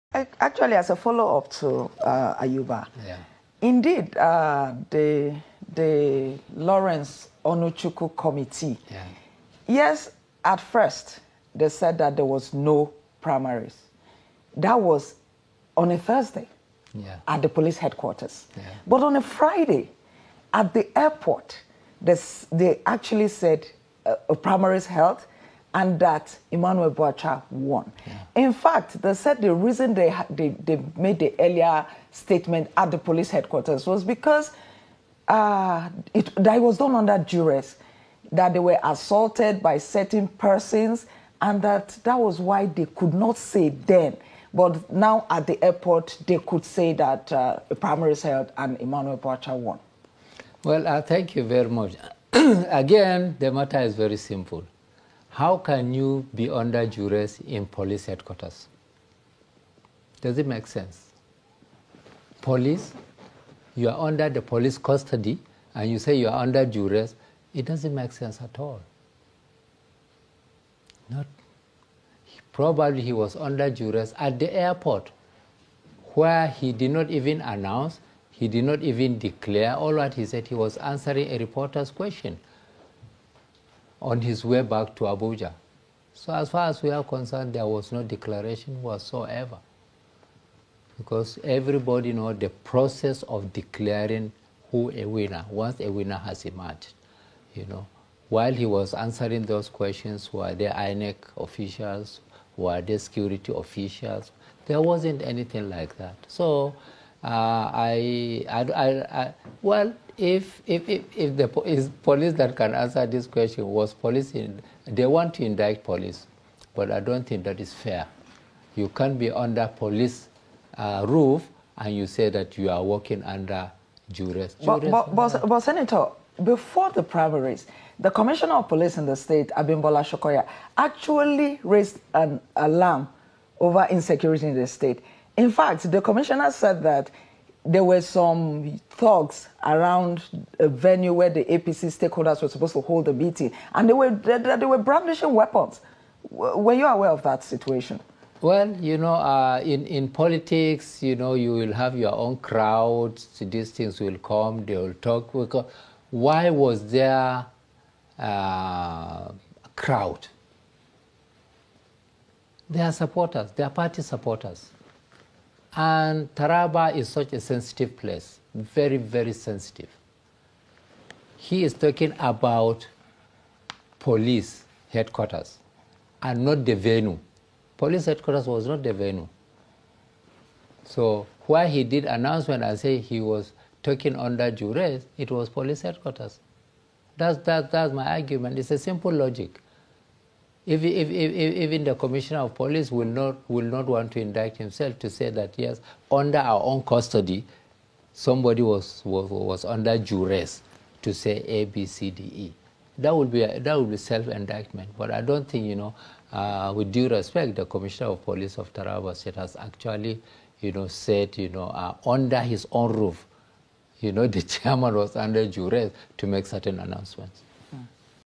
Sen YAY, on Trust TV, on Botched TR APC Guber Pri
Botched Taraba APC Gubernatorial Primary Election - Was Election Chairman, Chief Onuchukwu, under Duress at TR State Police Hqtrs? - Sen YAY on Trust TV.